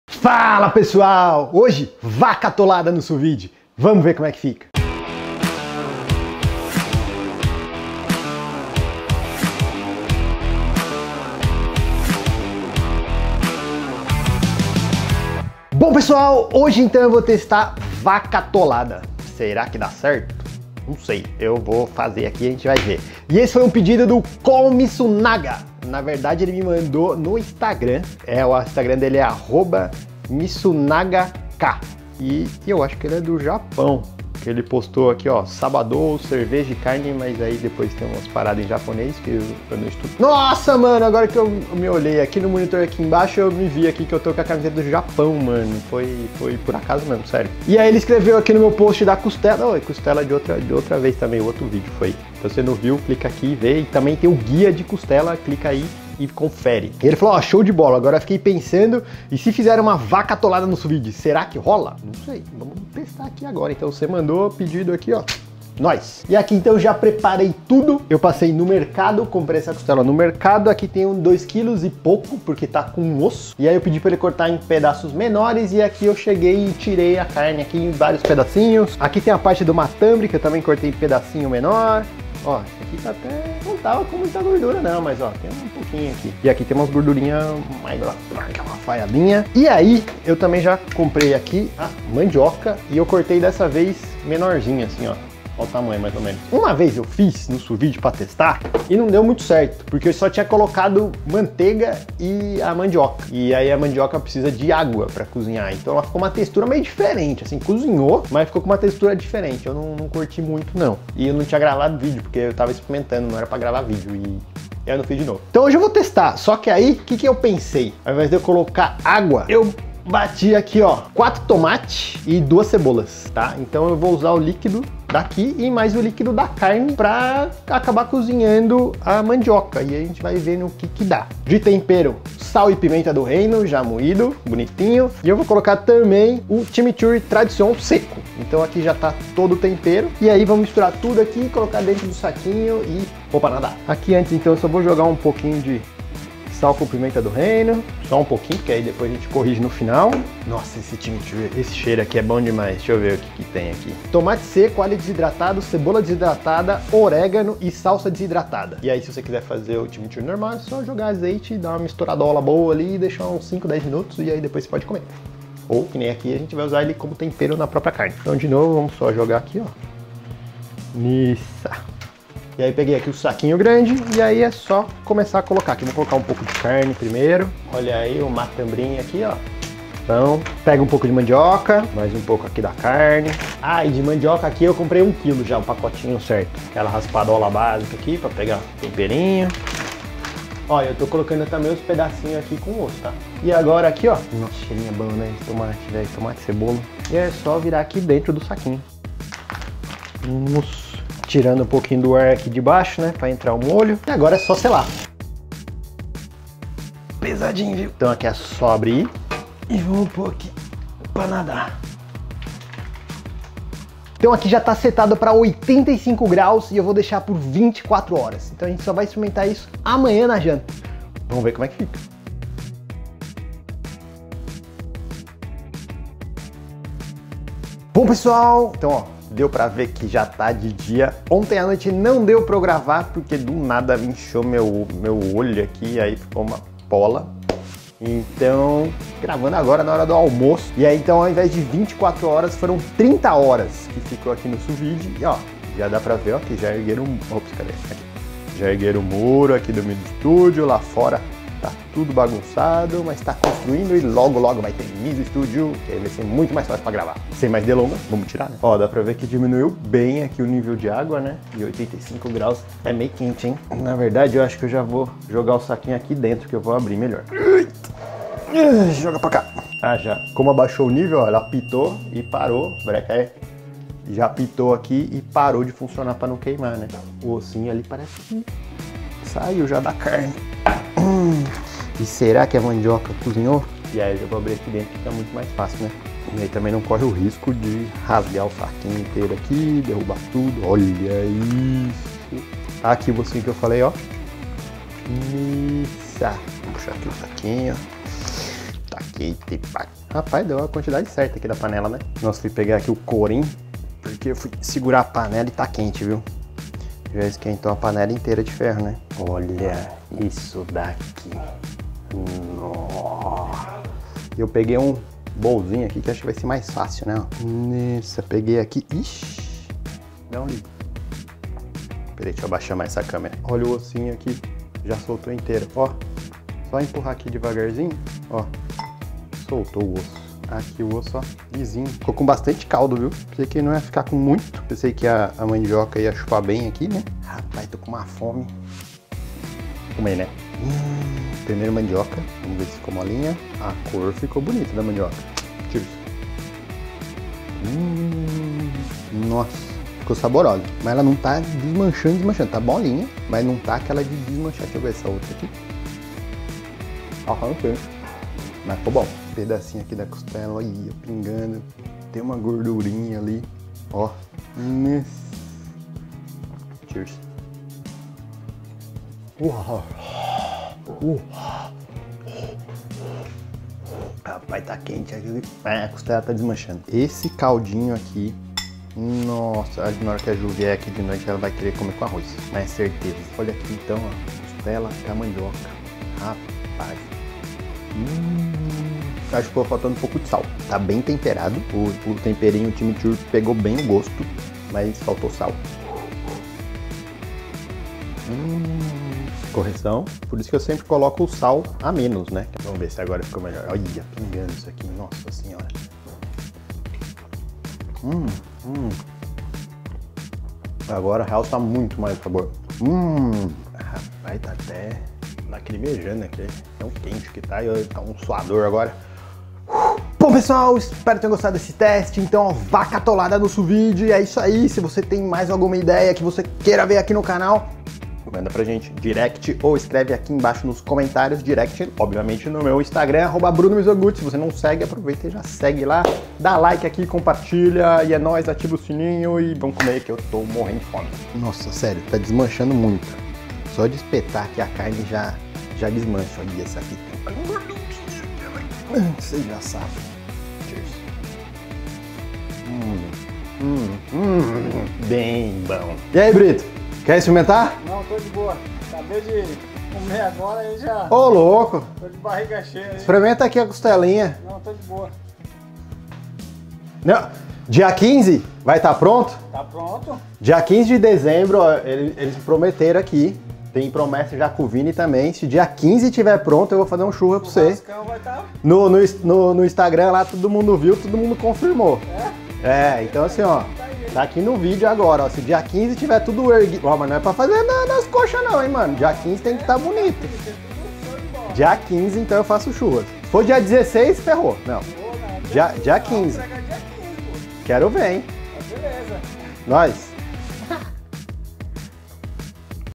Vaca atolada de 30H no Sous Vide! | Só Vide #260
Como fazer vaca atolada no sous vide!!!
Confere aeh! ;)